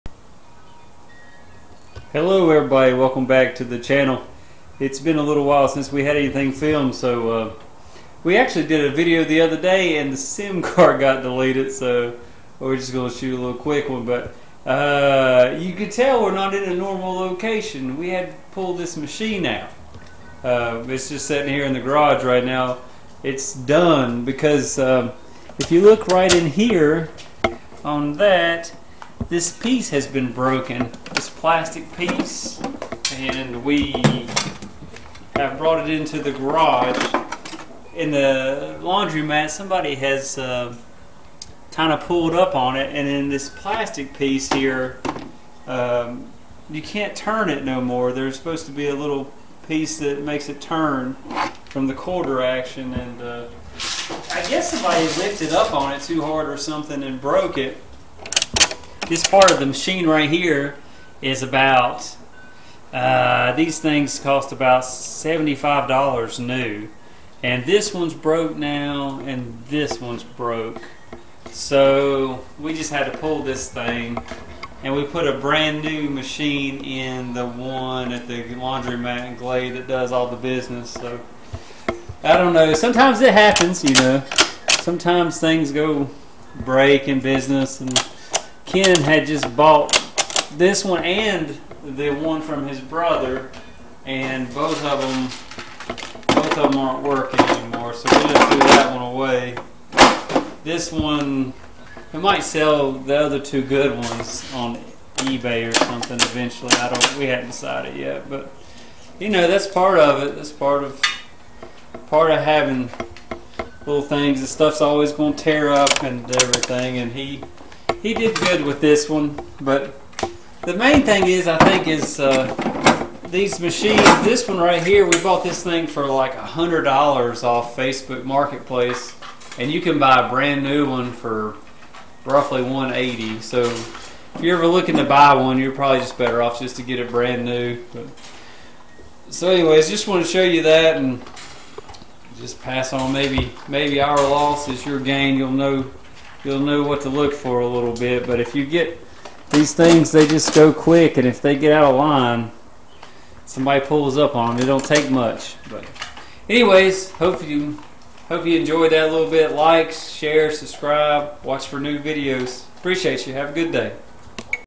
Broke down candy machine.
One of our used candy machines is ready to retire.